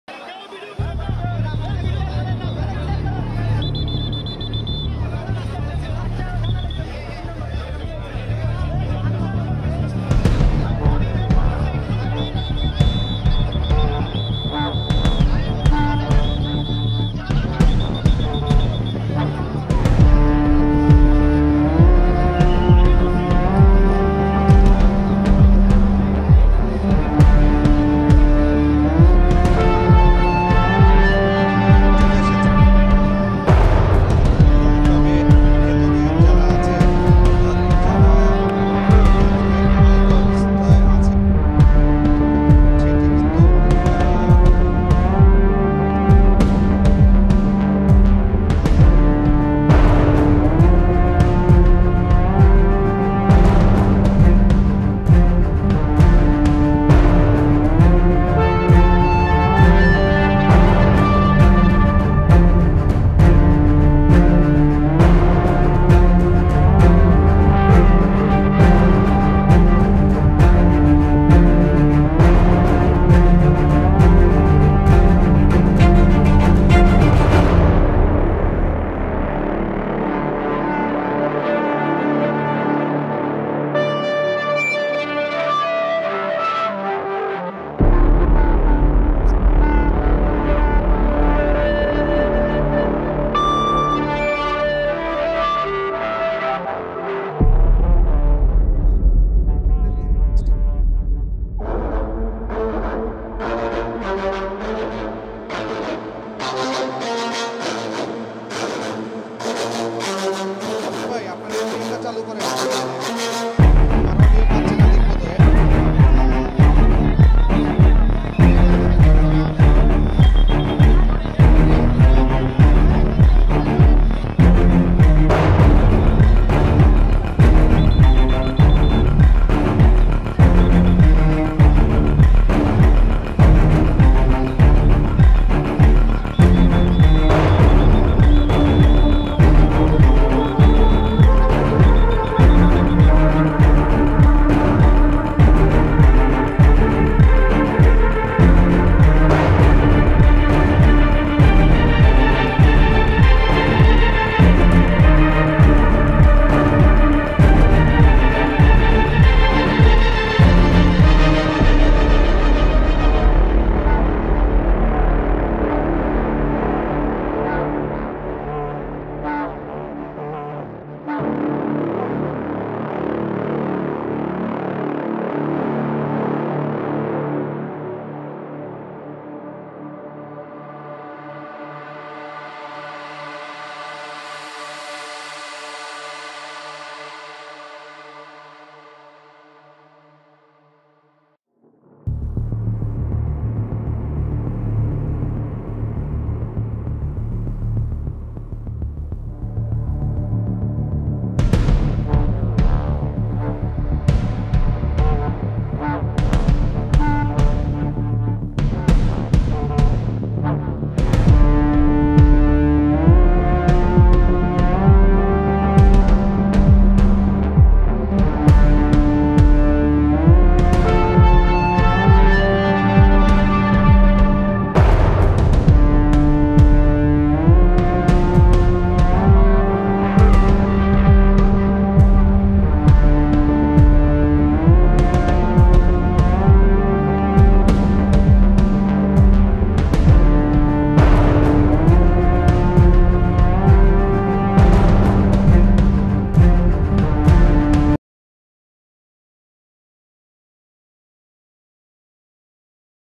বেনাপোল এক্সপ্রেস ট্রেনে আগুন; নিহত ৪ | Benapole express | Fire Service | RAB | Channel 24
»» Office Address:
Bangladesh.